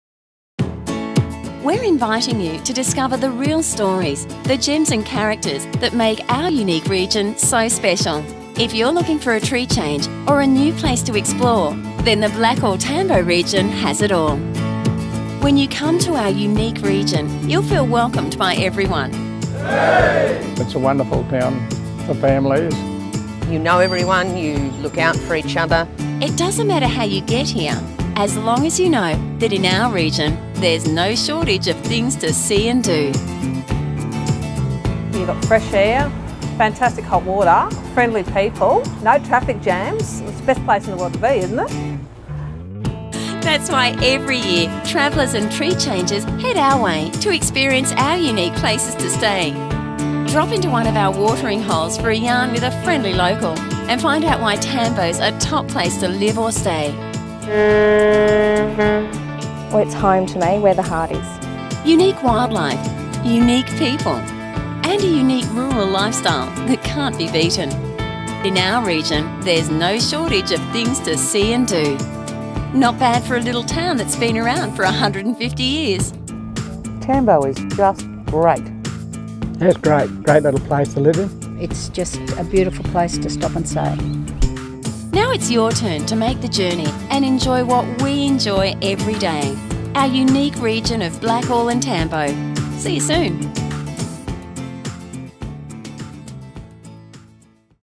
Our Unique Region - Blackall Tambo Queensland Australia
Queensland's Central West has been keeping a few things secret for a while now... but the Blackall - Tambo Region wants to share their treasures with everyone.

We're inviting you to discover the real stories, the gems and characters that make our unique region so special.

If you're looking for a tree change, or a new place to explore, then the Blackall - Tambo region has it all. Unique wildlife - unique people and a unique rural lifestyle that can't be beaten.

When you come to Our Unique Region you'll feel welcomed by everyone. Come for a visit and you'll stay forever.

The people who live here feel blessed to have found a thriving community built on hard work and determination.

The smiling, happy faces that greet you are always willing to chat, to help each other whether you're a local or not.

If you're strolling down Blackall's main street, quenching a thirst at our friendly pubs or enjoying our famous art and sculptures, you'll find the real treasures of Blackall. Our creative spirit shines in everything we do and you're more than welcome to join us in the fun.

It doesn't matter how you get here - by air or road - as long as you know that in our region there's no shortage of things to see and do.

That's why every year traveller's and 'tree-changers' head our way to experience our unique places to stay.

Our stories are spoken and sung to you by our locals wherever you go.

We're truly proud of our place and were happy to tell you all about it.

Blackall is a place for the young and not so young. There's something for everyone here.

Our region is renowned for our wealth of produce. We're the home of world class cattle and sheep that are the heart of our vibrant economy.

Our natural heritage is vast and diverse too. When you get out here you'll be amazed at Blackall's neighbouring National Parks and our stunning native animals and plants.

Another great reason people travel here is national treasure and local shearing legend Jackie Howe.

Just a short trip from Tambo and you'll discover the Wilderness Way.

It's part of Sir Thomas Mitchell's original track through the region and meanders around the scenic views of the Great Dividing Range, Salvatore Rosa National Park and back to Tambo. You mustn't miss it!

It's amazing how much the landscape changes along the way.

Just park up for free camping in Tambo and enjoy the many walks around the place. You'll soon find out why Tambo - the oldest town in the Central West - was originally settled and became famous for it's wool and nowadays for its handmade woollen teddy bears.

It didn't take long for our community to grow into the great little town that's full of welcoming faces.

Drop into one of our watering holes for a yarn with a friendly local and find out why Tambo's a top place to live or stay, whether it's for a lifetime or a picnic in the park.

At first, Tambo may seem like a sleepy little village but there's plenty to see and do here too.

Tambo comes to life on race days, polocrosse meets - in fact if you're adventurous you'll find your place soon enough. There's just so much room to move - just saddle up and start exploring!

Locals remember the fateful day of Australia's first and only QANTAS crash here on Tambo's own claypan...

Tambo has a proud history that can be seen throughout our heritage precinct. The court house, post office and telegraph station are great places to experience our history first hand.

Well what do you reckon??? Not bad for a little town that been around for 150 years'.

Well, we've got to leave you now. It's all up to you! Now it's your turn to make the journey and enjoy what we enjoy everyday - our unique region of Blackall and Tambo... See you soon!

BTW, no bears were harmed in the making of this film...